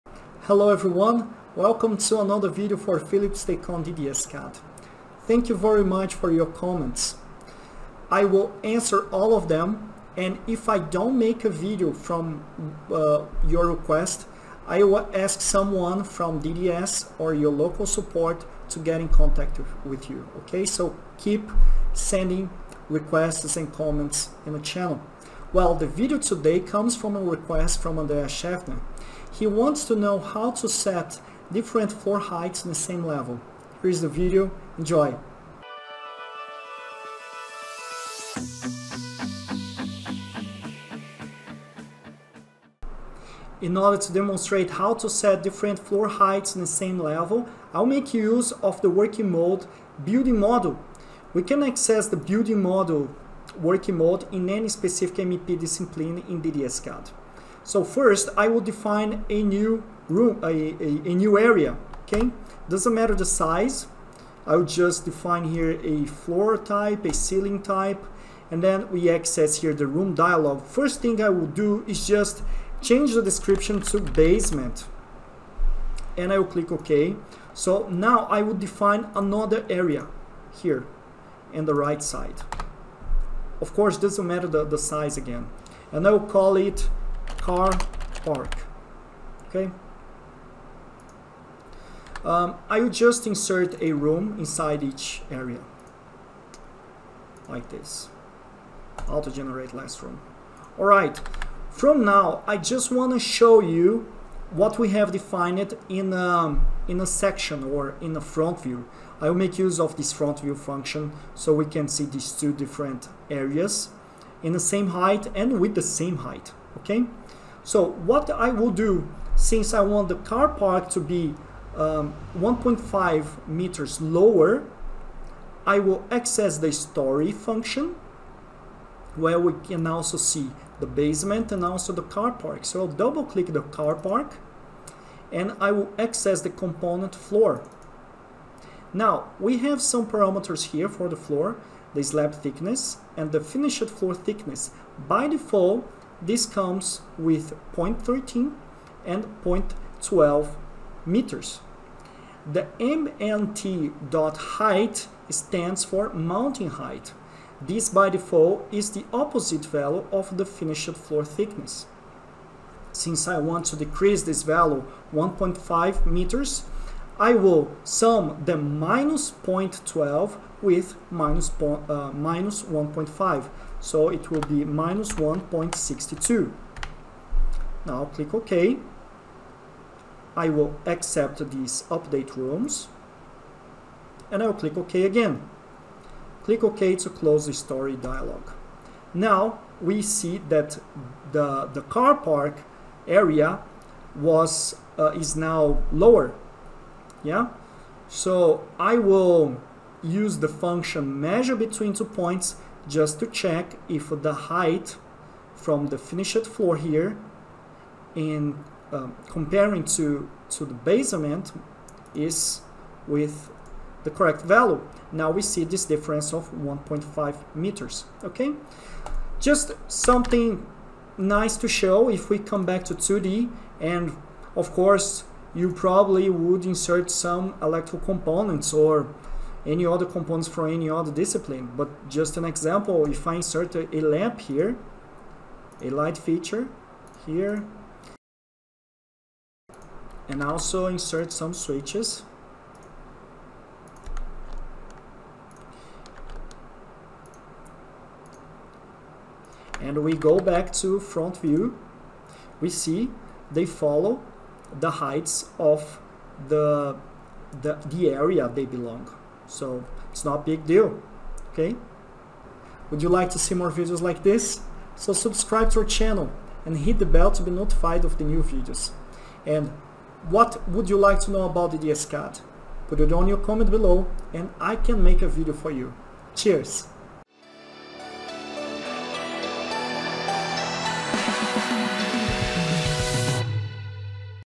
DDScad | 📢 How do I set different floor heights in the same level? 🤔🧐🤯
We received a comment asking how to set different floor heights in the same level, so in this video Filipe demonstrate in a simple sample how you can easily do it.